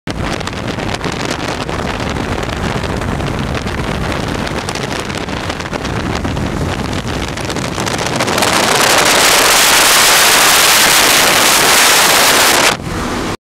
B-17 - Waist Open Air Top Of Plane View - 172mph winds
150kt (172mph) winds!  I know the winds were this speed as I have a still shot photo of the indicated airspeed showing 150 knots!  For those who know aviation terminology, indicated airspeed isn't the same as true airspeed or speed over ground.